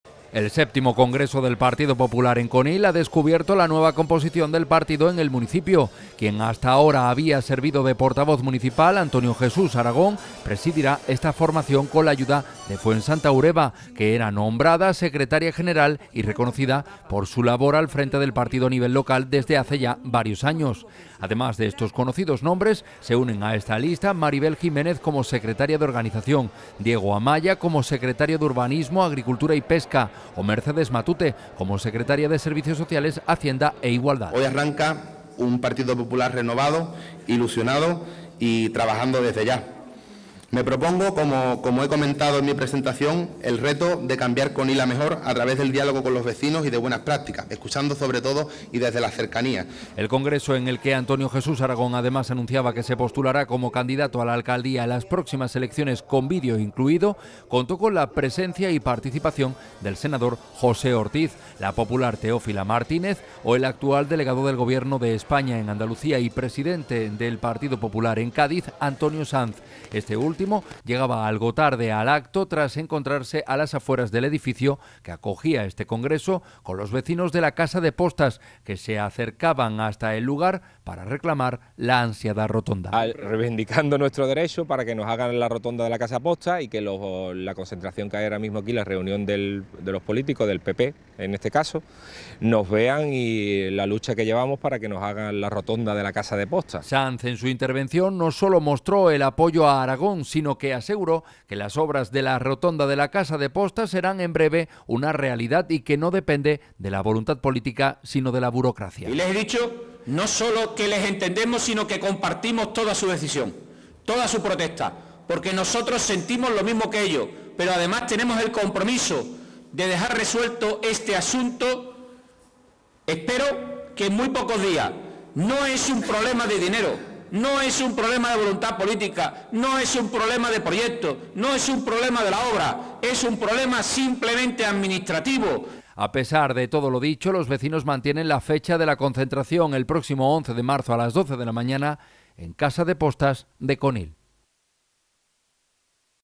8NOTICIAS JANDA - 26FEB2018
Antonio Jesús Aragón era elegido este fin de semana en el congreso local del Partido Popular de Conil que contó con el apoyo de Antonio Sanz o Teófila Martínez.